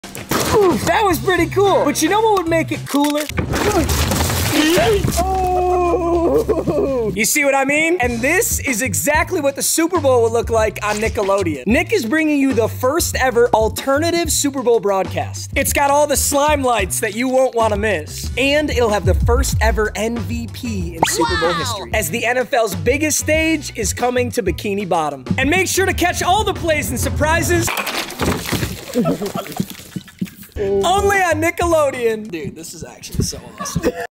The Best Way To Watch Super Bowl LVIII
ad Boys there’s a new way to watch Super Bowl LVIII! Make sure to catch all the slimelights, only on @Nickelodeon